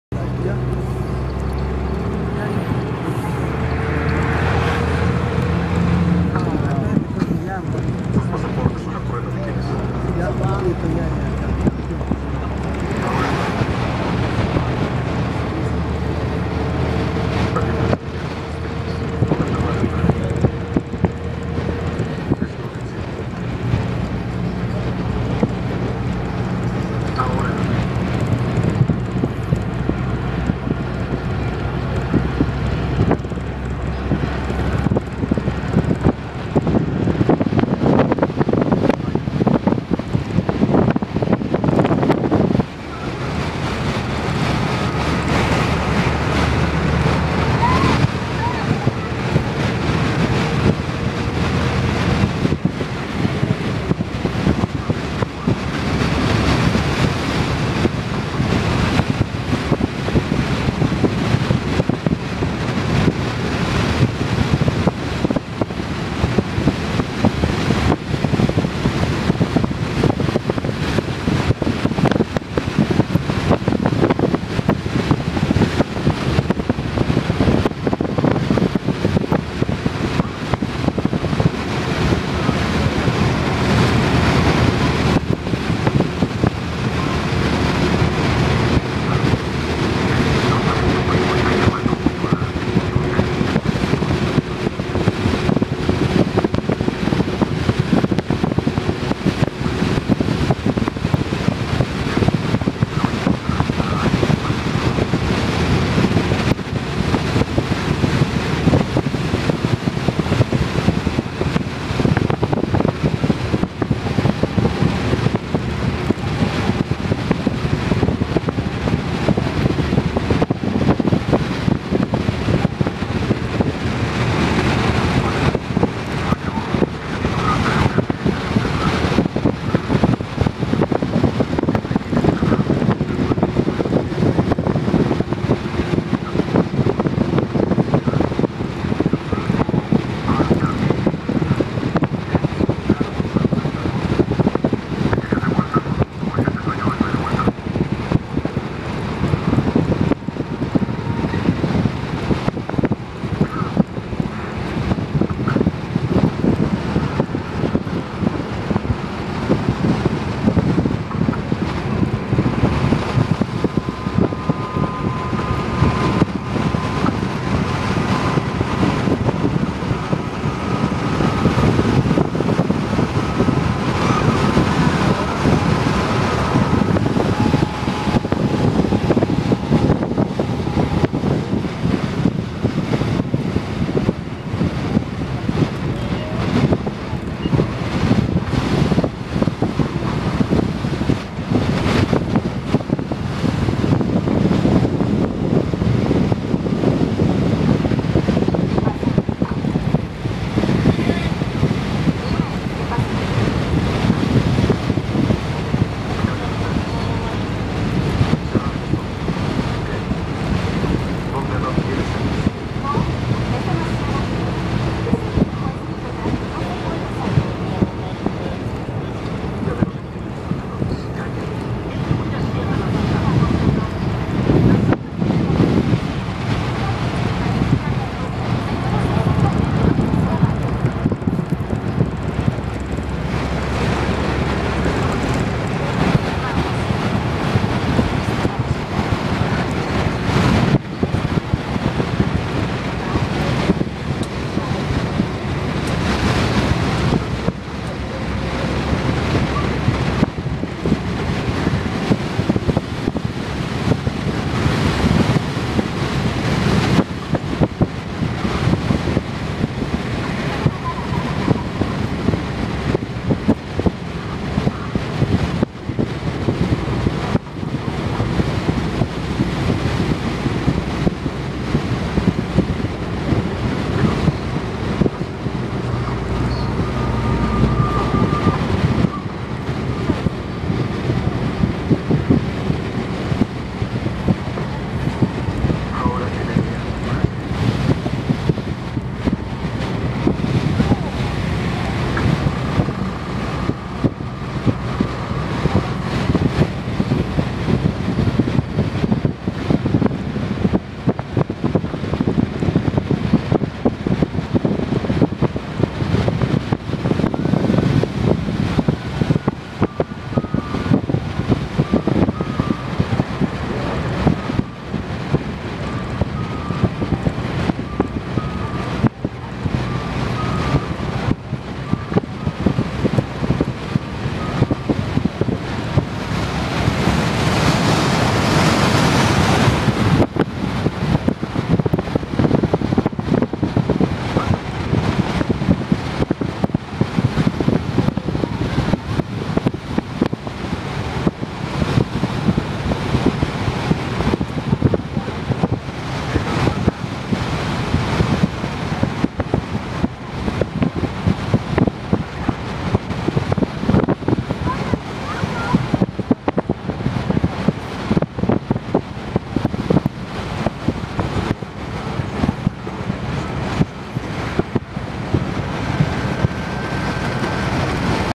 Nueva Autopista Huarmey - Casma 1
Este video muestro la nueva calzada del Tramo Huarmey a Casma en Ancash recien inaugurada.
Pronto nueva autopista Pativilca a Huarmey que esta en construccion no lo pude filmar ya tenia poca bateria espero que sea de su agrado.
Peru